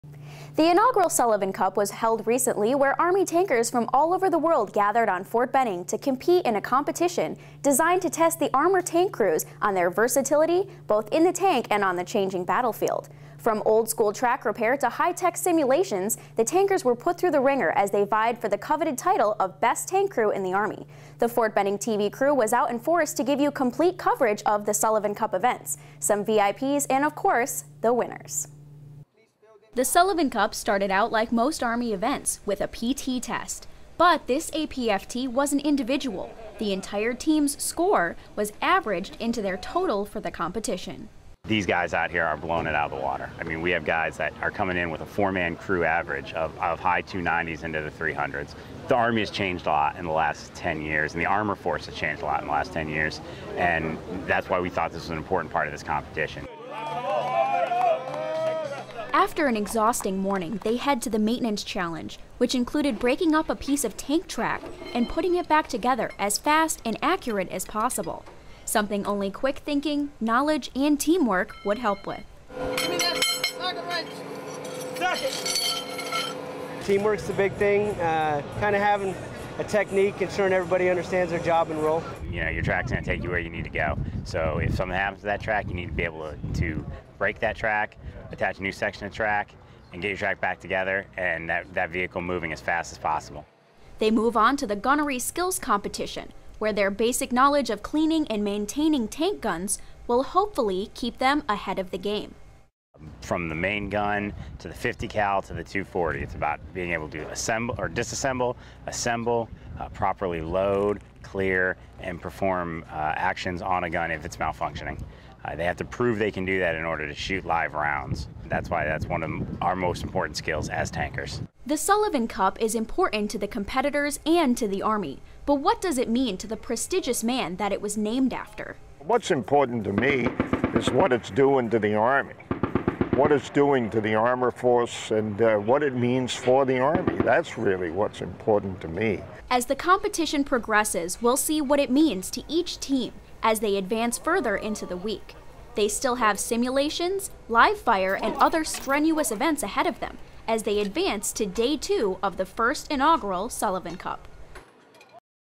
Sullivan Cup Day 1 Fort Benning
The inaugural Sullivan Cup was held recently where Army tankers from all over the world gathered on Fort Benning to compete in a competition designed to test the Armor tank crews on their versatility both in the tank and on the changing battlefield. From old school track repair to high tech simulations, the tankers were put through the ringer as they vied for the coveted title of best tank crew in the Army. The Fort Benning TV crew was out in force to give you complete coverage of Sullivan Cup events, some VIPs and of course the winners.